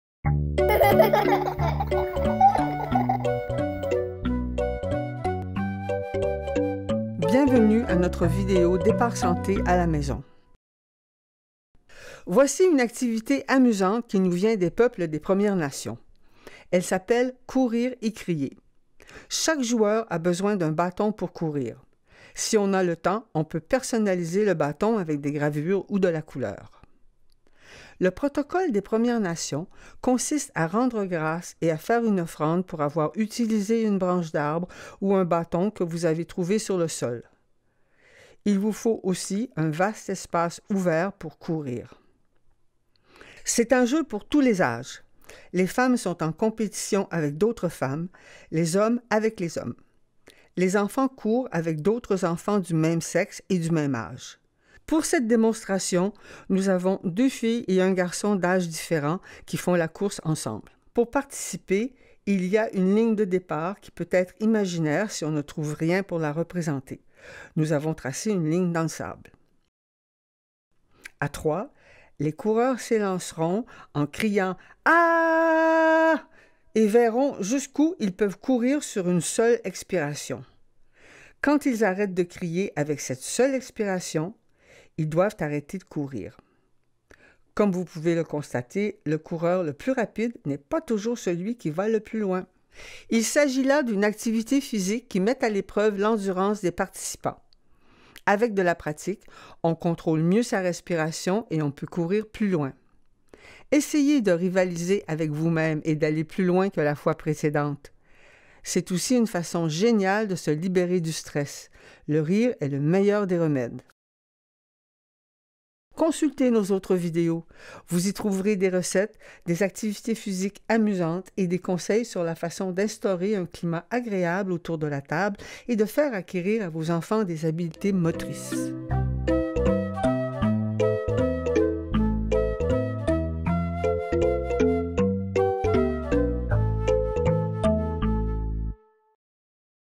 Départ Santé @ la maison : Le jeu Courir et crier
Un jeu qui nous a été transmis par les Premières Nations